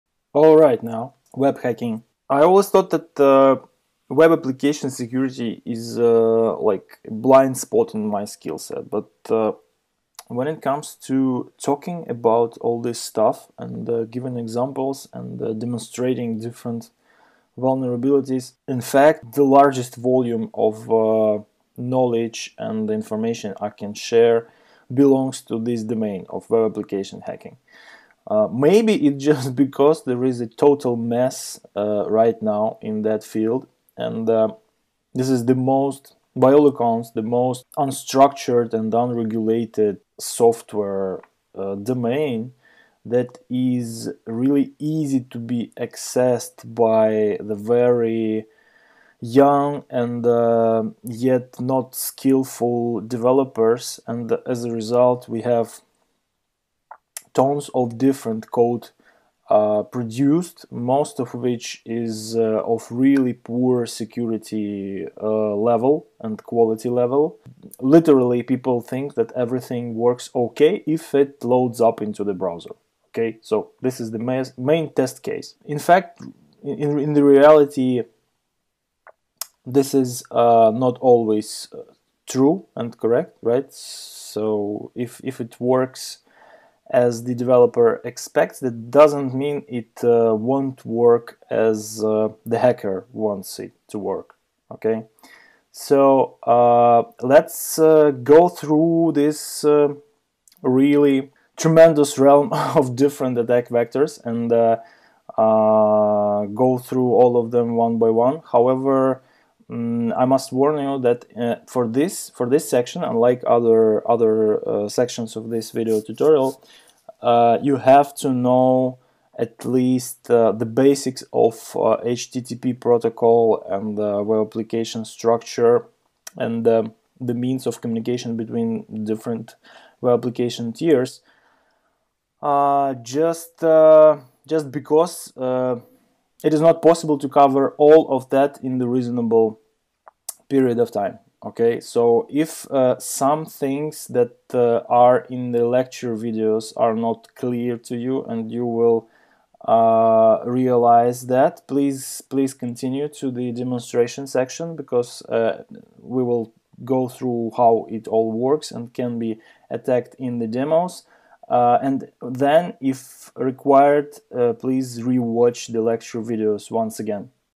61 Introduction to web hacking
Ethical hacking and ethical hacker are terms used to describe hacking performed by a company or individual to help identify potential threats on a computer or network. An ethical hacker attempts to bypass system security and search for any weak points that could be exploited by malicious hackers